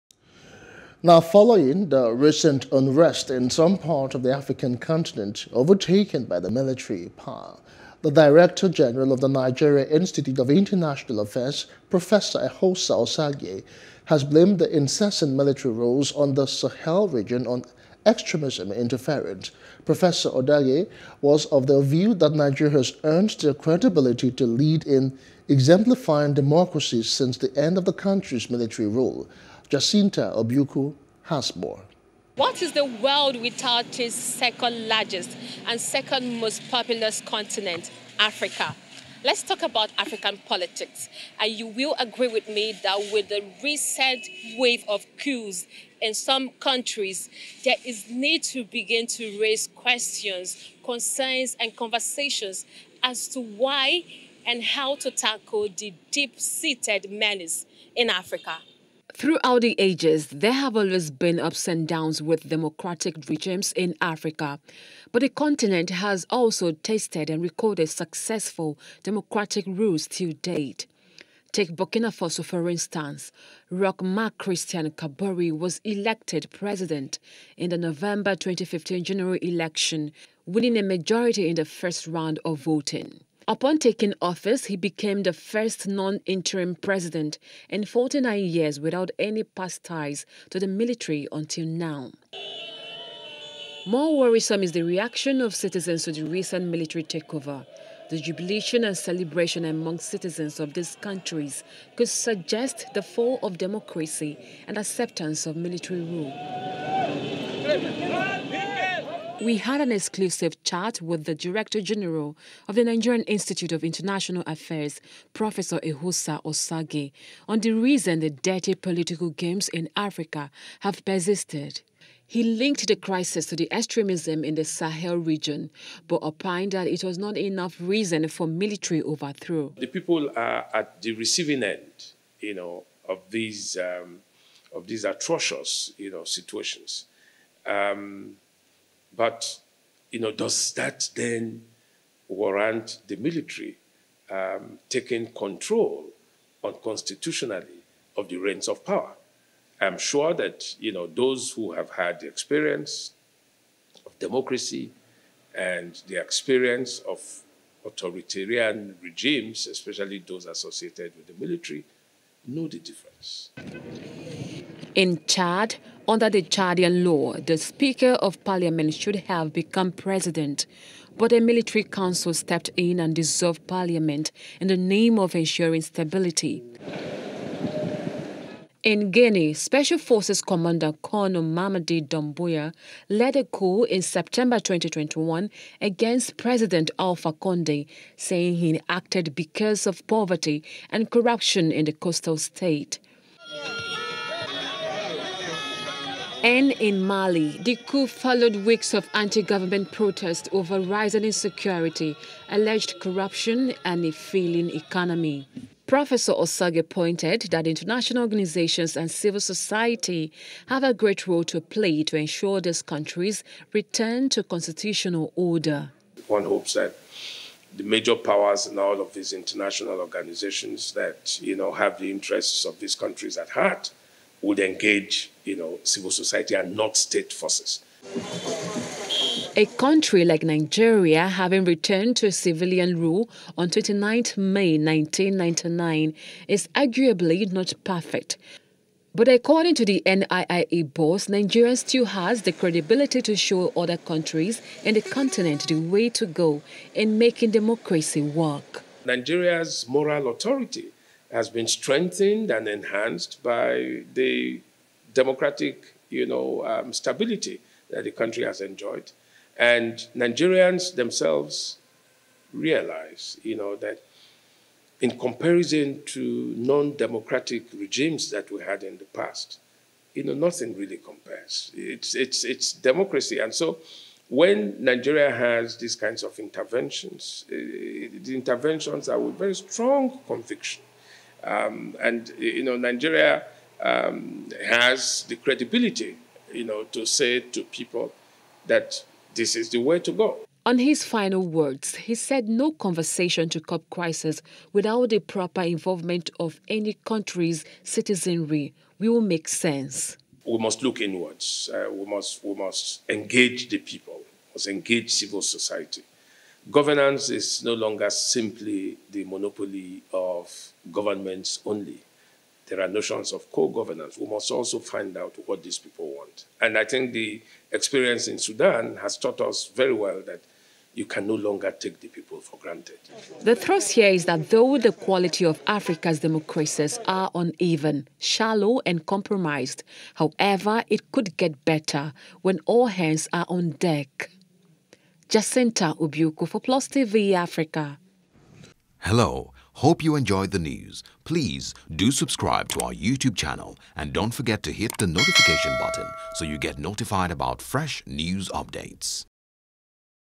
African Politics: Not Enough Reasons Justify Coups - D.G., N.I.I.A | NEWS
Following the recent unrest in some part of the African continent, overtaken by the military power, the Director general of the Nigeria institute of international affairs Prof. Eghosa Osaghae has blamed the incessant military rules on the Shahel region on extremism interference